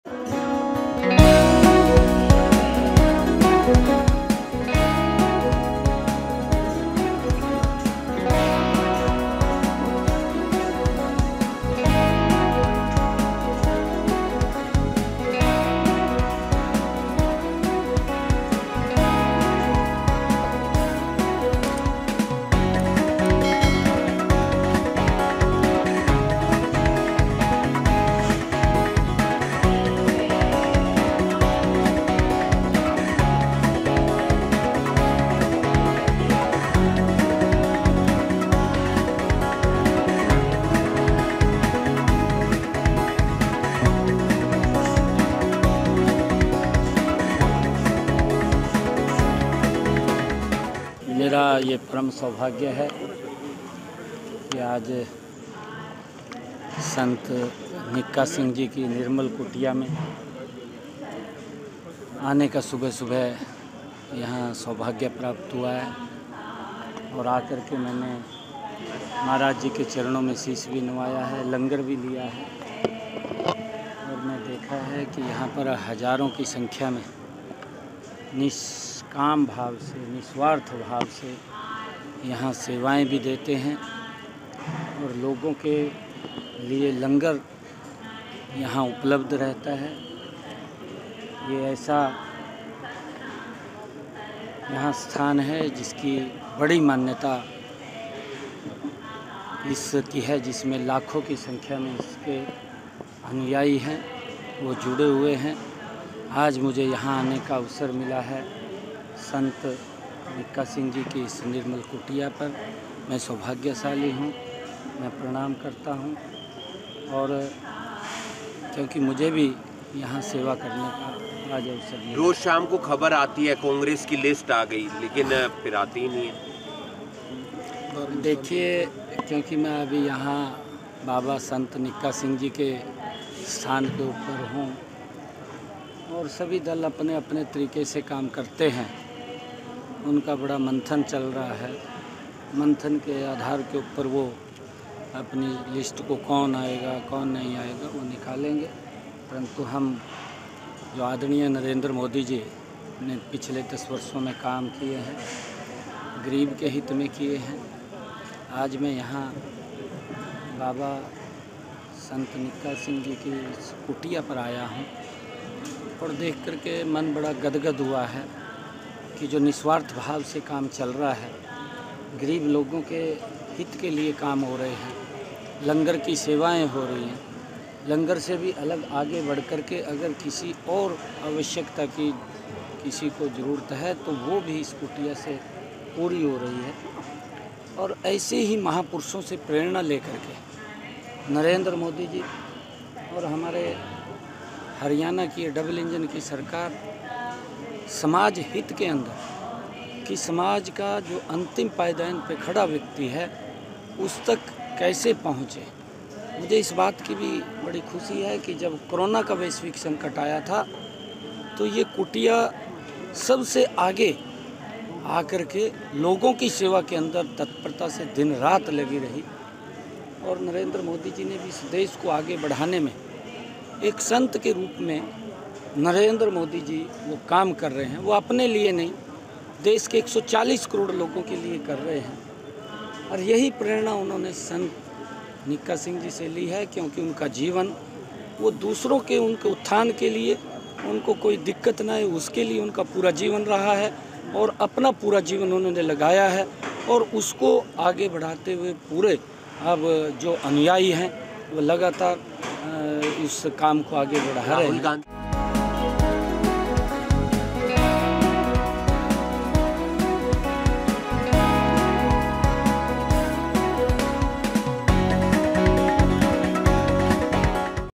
Haryana CM Nayab Singh Saini takes blessings of Sant Nikka Singh in Karnal
Haryana CM Nayab Singh Saini visited Nirmal Kutia of Sant Nikka Singh in Karnal on April 16. Haryana CM thanked the Nirmal Kutia for its public service on daily basis and especially during the COVID lockdown. He spoke on the upcoming Lok Sabha elections and said that BJP is sure of a big win under the leadership of Prime Minister Narendra Modi.